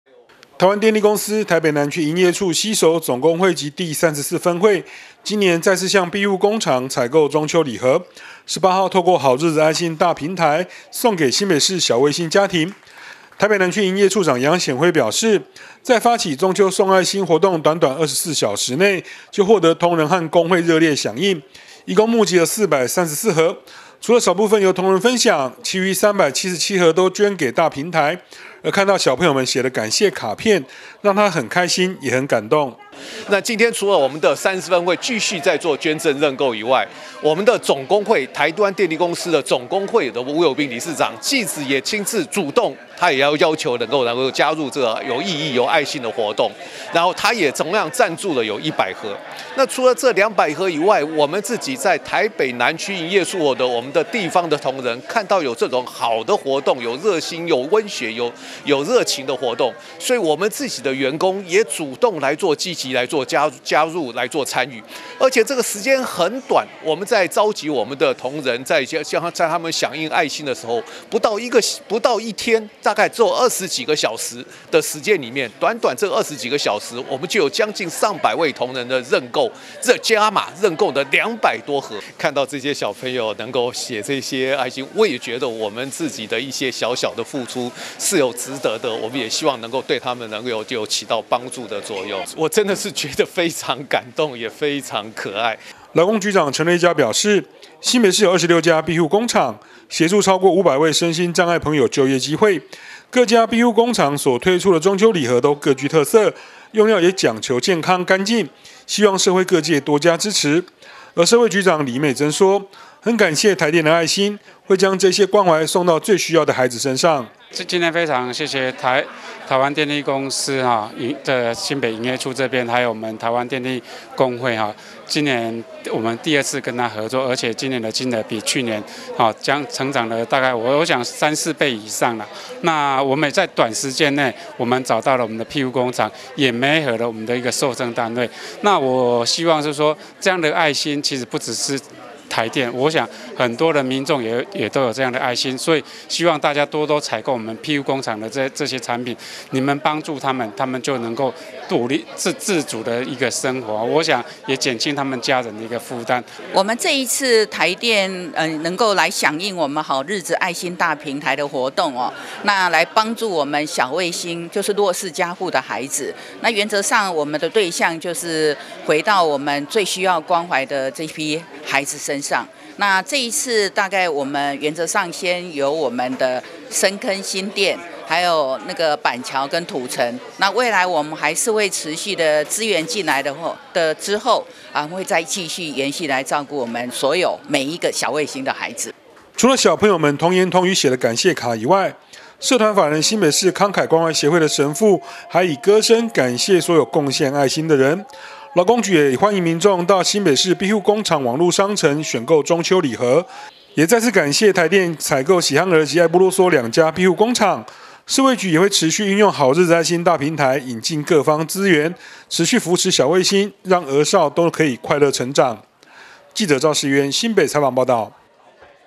庇護 公益|台電台北南區營業處 認購庇護工場禮盒贈小衛星
台電台北南區營業處連續2年響應新北市好日子愛心大平台，今年與台灣電力工會吳有彬理事長及第34分會共同攜手，再次向庇護工場採購377份中秋禮盒，送給新北市小衛星的弱勢家庭。台北南區營業處長楊顯輝表示，台電向來秉持「人飢己飢，人溺己溺」理念，善盡企業社會責任，展現企業力共同為社會帶來更多溫暖。

台灣電力公司台北南區營業處攜手總工會及第34分會，今年再次向庇護工場採購中秋禮盒，18日透過好日子愛心大平台送給新北市小衛星家庭，台北南區營業處長楊顯輝表示，在發起中秋送愛心活動短短24小時內，就獲得同仁和工會熱烈地響應，共募集了434盒，除了少部分由同仁分享，以示支持庇護工場外，其餘377盒均捐給大平台，而看到小朋友們寫的感謝卡片，讓他很開心也很感動。

勞工局長陳瑞嘉表示，新北市有26家庇護工場，協助超過500位身心障礙的朋友就業機會，各家庇護工場所推出的中秋禮盒都各具特色，用料也講求健康、乾淨，希望社會各界多加支持，社會局長李美珍說，很感謝台電的愛心，會將這些關懷送到最需要的孩子身上。

除了小朋友們童言童語寫的感謝卡以外，社團法人新北市慷慨關懷協會的神父還以歌聲感謝所有貢獻愛心的人，勞工局也歡迎民眾到「新北市庇護工場網路商城」選購中秋禮盒，再次感謝台電採購喜憨兒及愛不囉嗦2家庇護工場，社會局也會持續運用好日子愛心大平台引入各方資源，持續扶持小衛星，讓兒少都可以快樂成長。